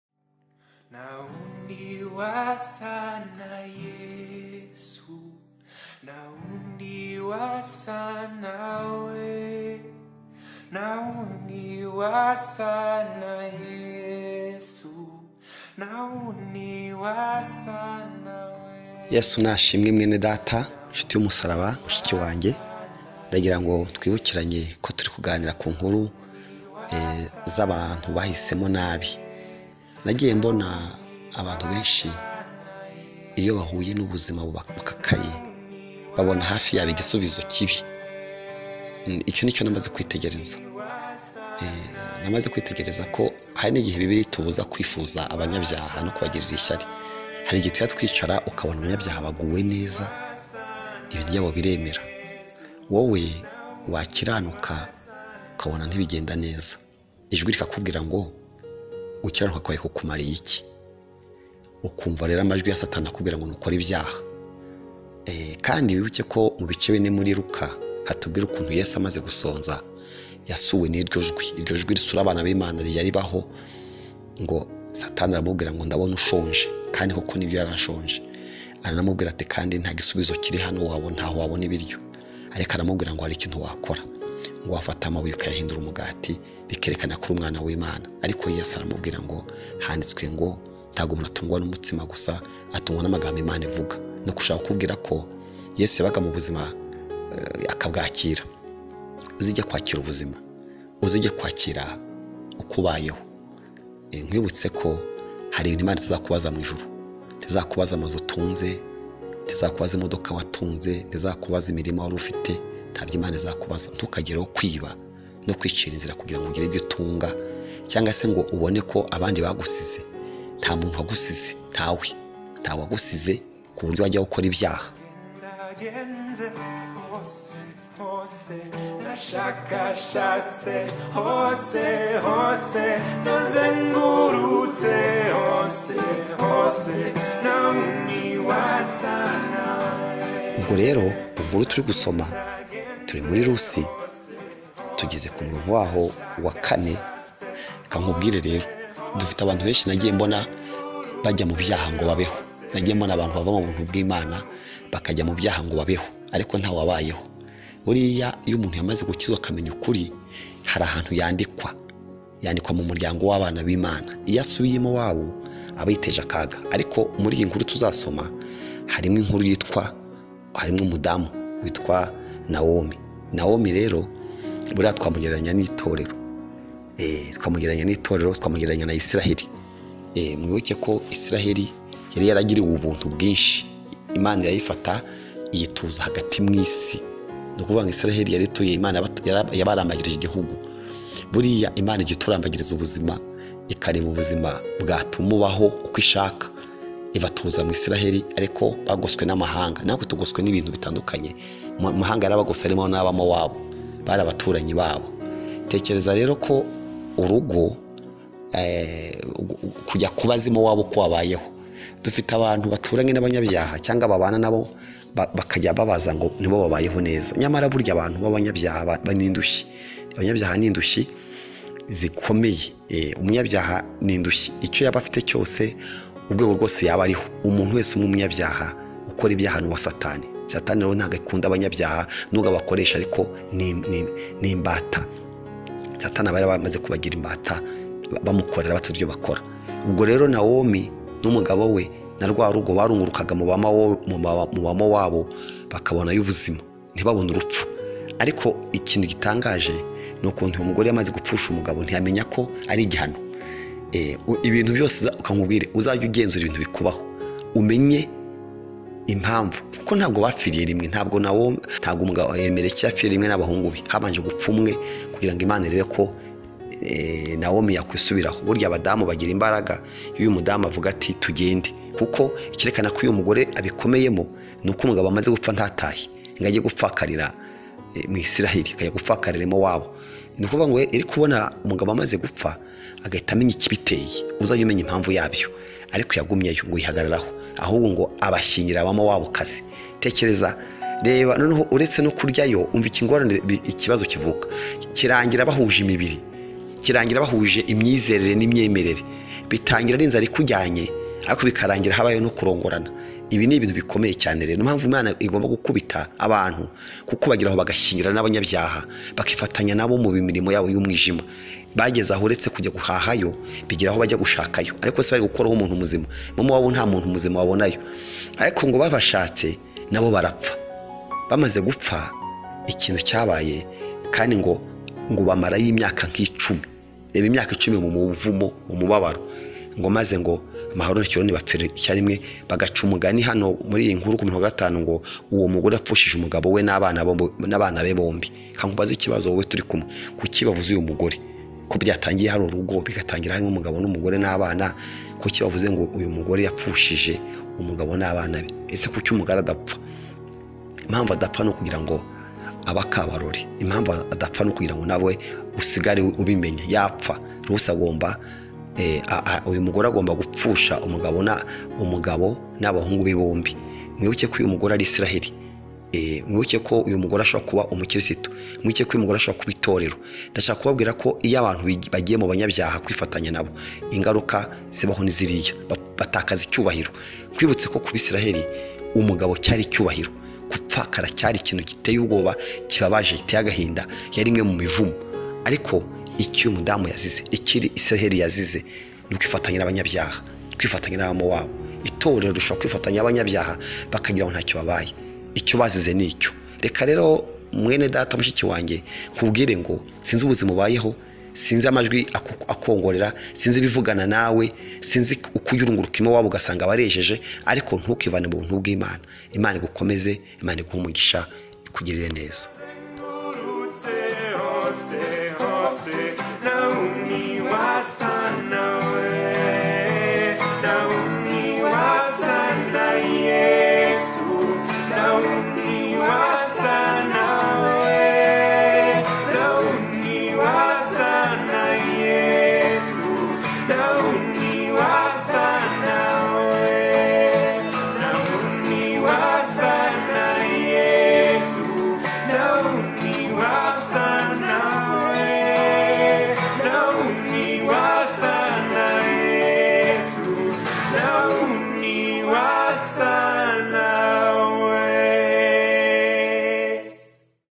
NTUKIFUZE IBY'ABANYABYAHA (Bikubere uko wizeye) | Pastor UWAMBAJE Emmanuel | 10/9/2021.
RUSI 1
4. Barongora Abamowabukazi, umwe yitwaga Orupa, undi yitwaga Rusi, bamarayo imyaka nk’icumi.
5. Maze Mahaloni na Kiliyoni bombi barapfa. Uwo mugore yapfushije umugabo we n’abana be bombi.